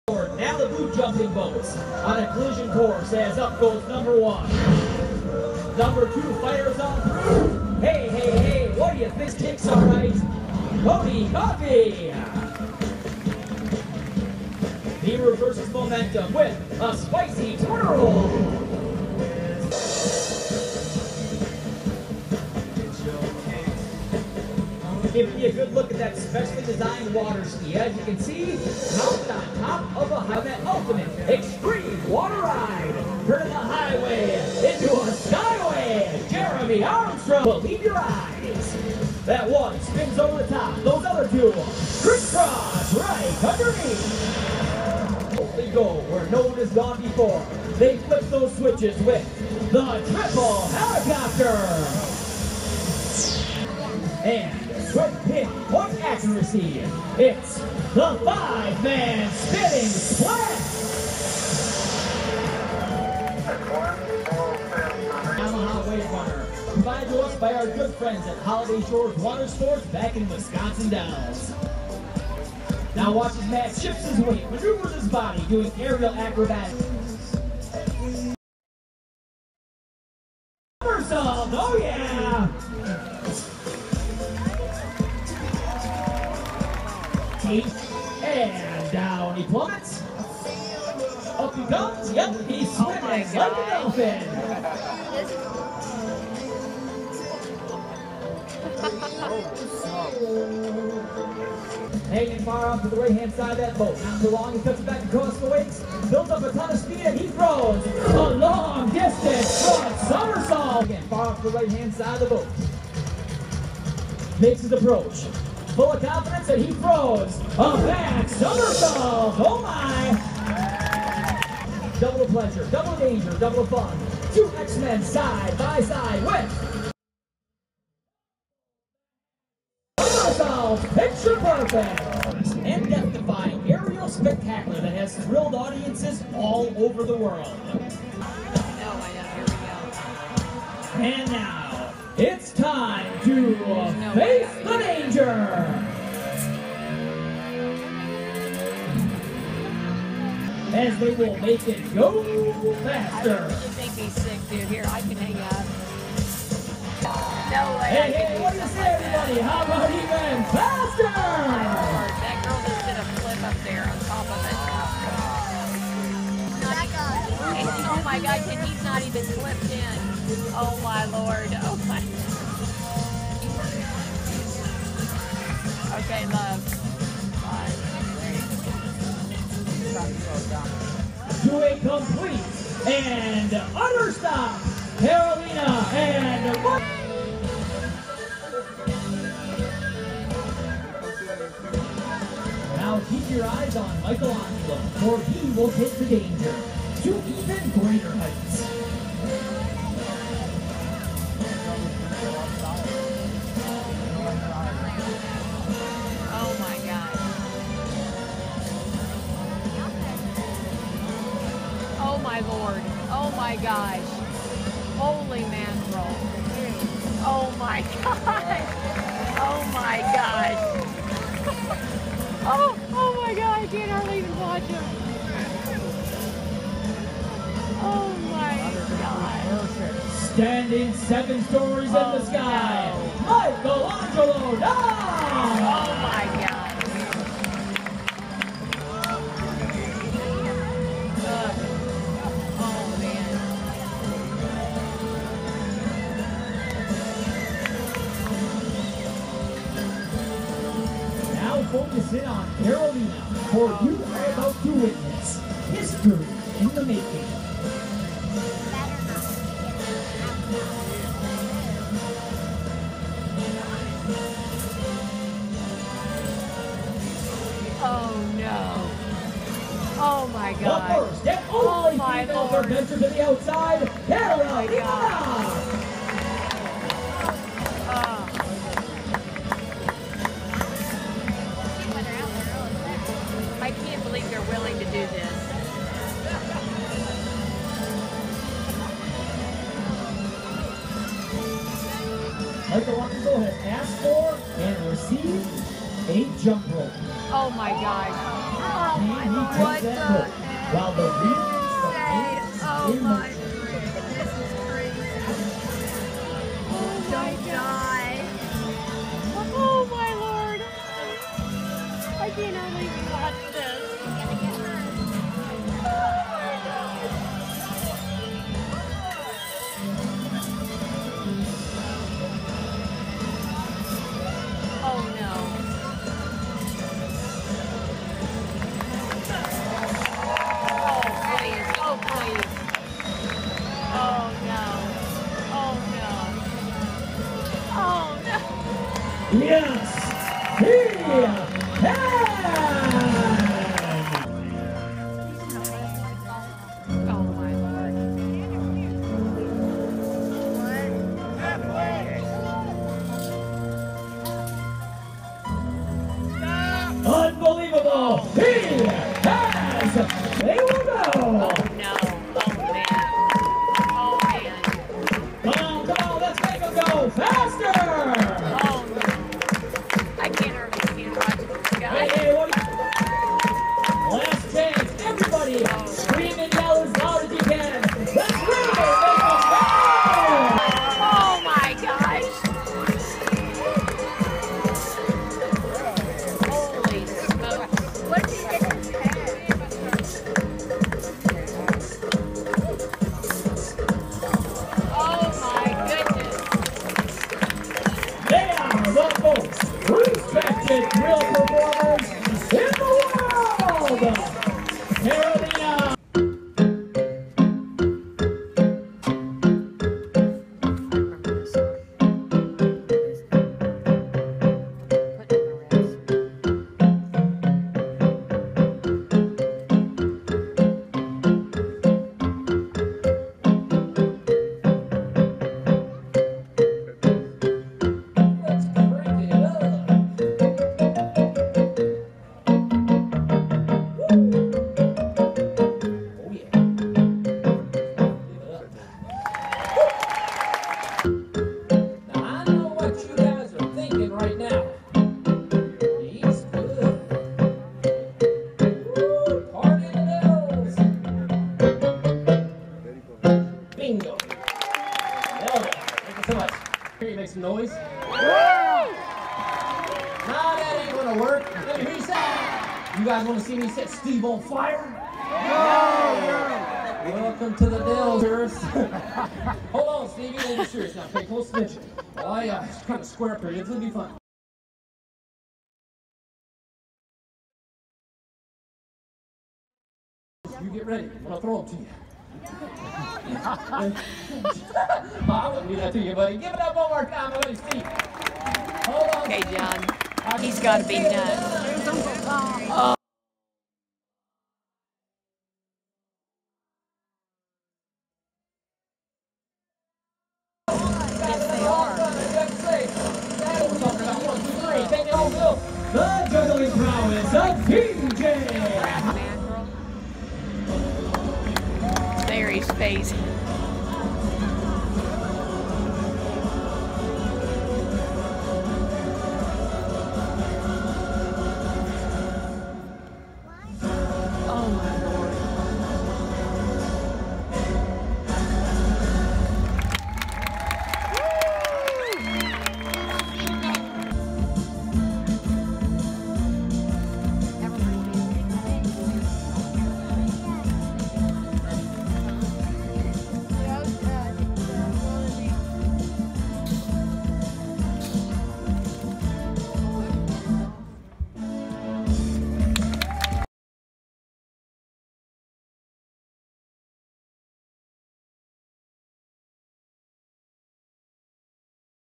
Tommy Bartlett Show 7/8/17!!
When my family was on vacation in the Wisconsin Dells, we went to the Tommy Bartlett Water ski and stunt show!! It was absoultely incredible and insane!! So many crazy and skilled acts! If you go the Dells, this is highly recommended 👍